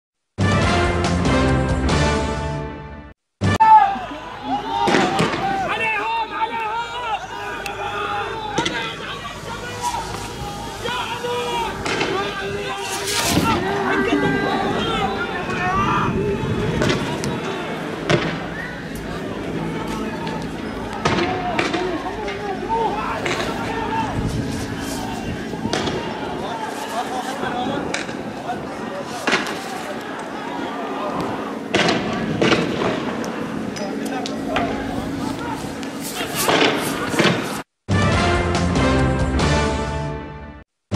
شاهد المرتزقة يجن جنونها .. وحالة هستيريا بعد وقوع الإنفجار في تقاطع مجمع الهاشمي Bahrain
3 مارس 2014
جدحفص